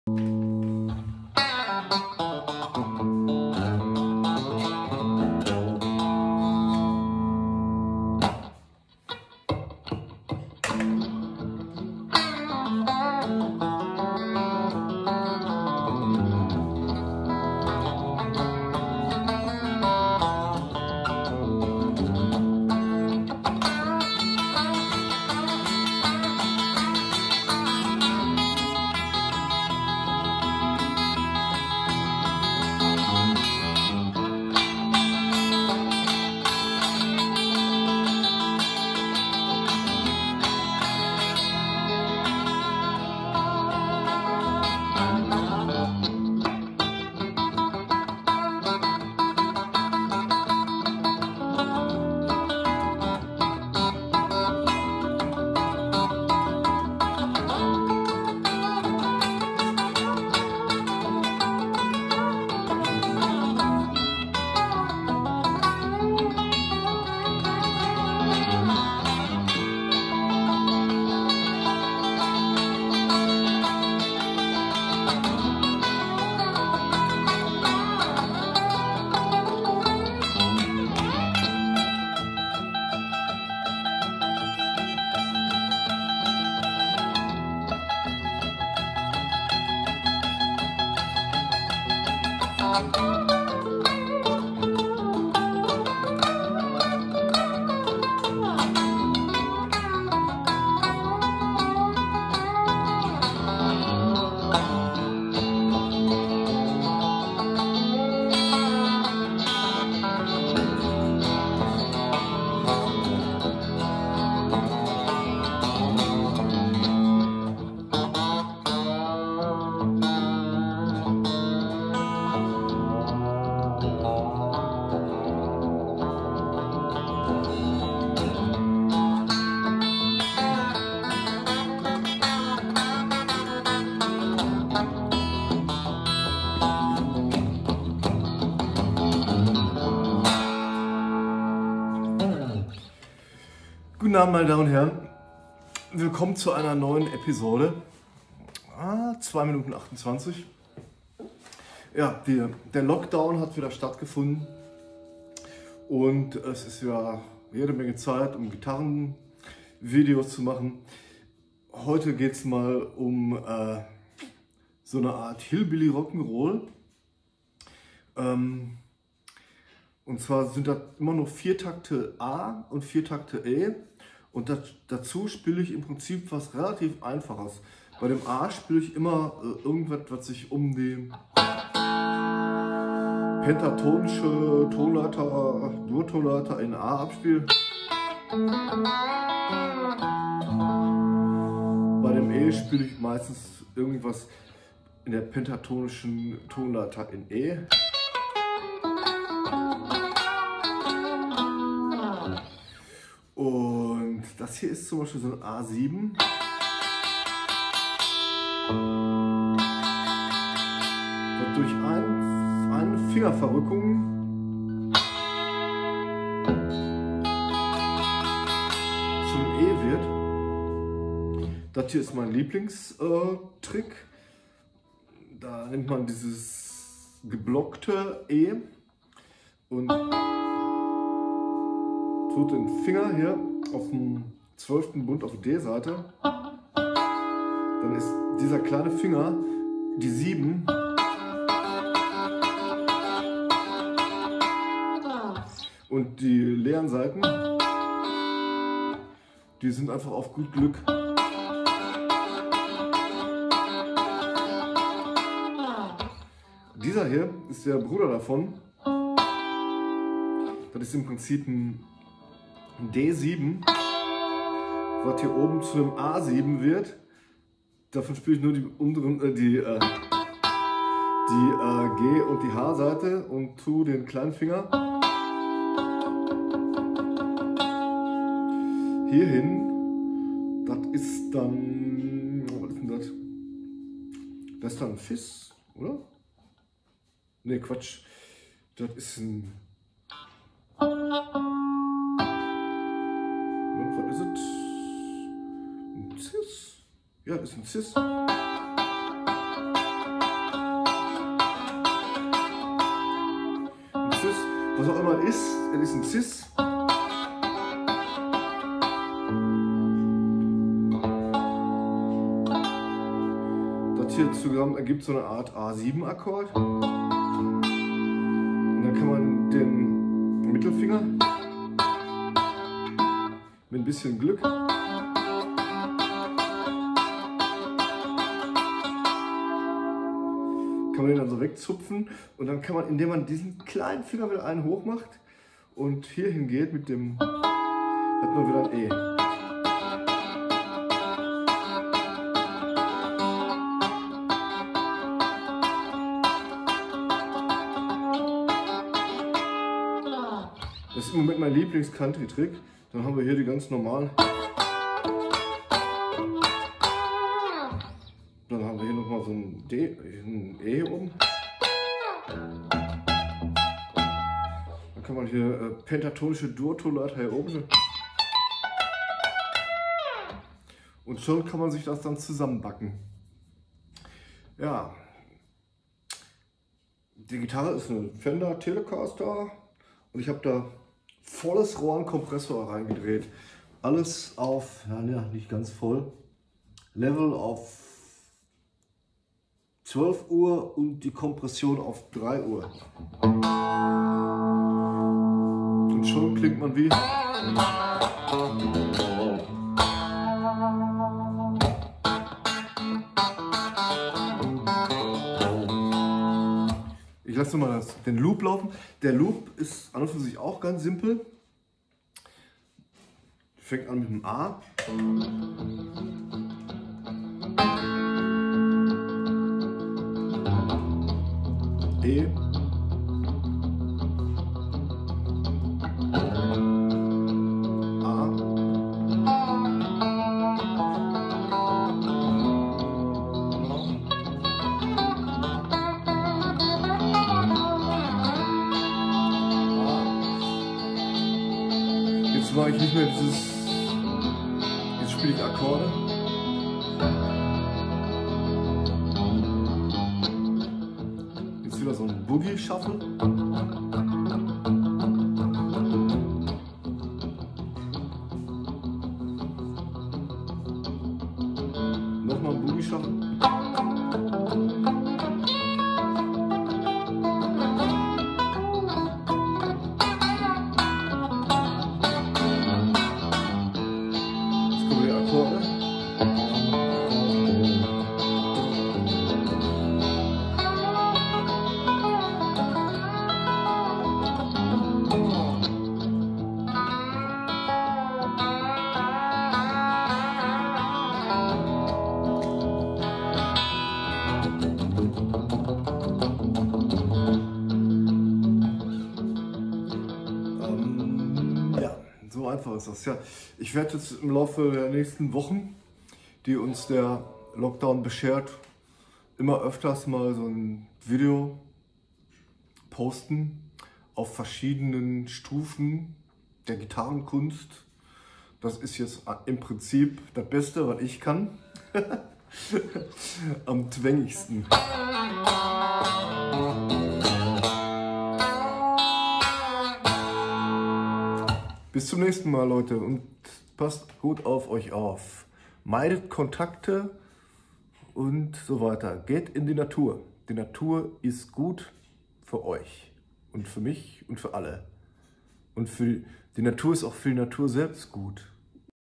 Superclean Telecaster Hillbillyrock
Nashville im Kohlenpott: Countrygedengel auf der total cleanen Gitarre.